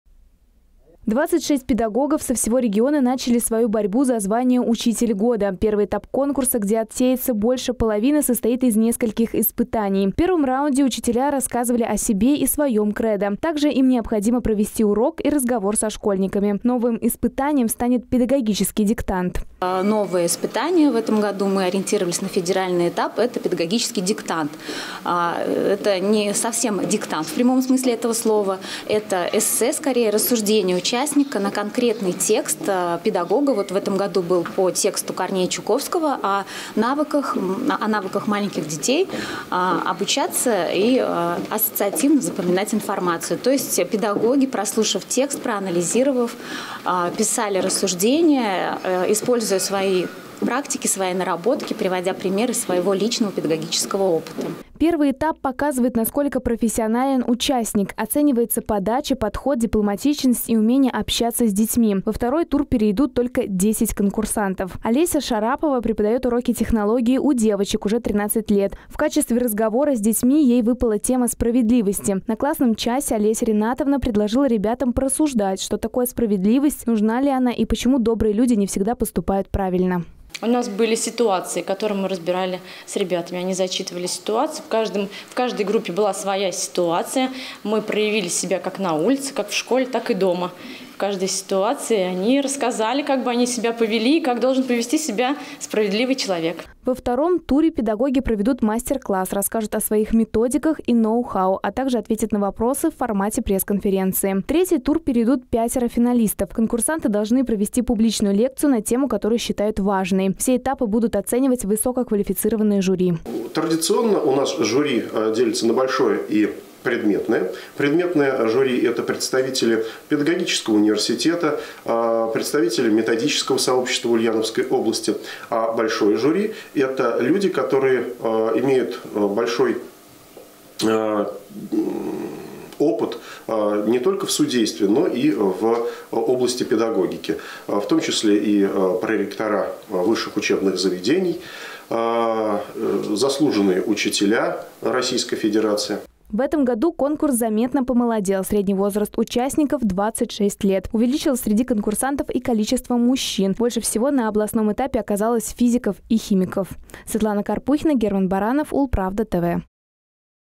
Больше мужчин и молодежи. Кто хочет стать «Учителем года» в Ульяновске
Ежегодный Всероссийский конкурс «Учитель года» традиционно начинается с муниципального этапа. Победители с районов области попадают на региональный этап. Здесь же и разгорается настоящая борьба, из 26 участников победителем окажется только один. Как проходит конкурс «Учитель года» в этом году, узнавала наша съёмочная группа.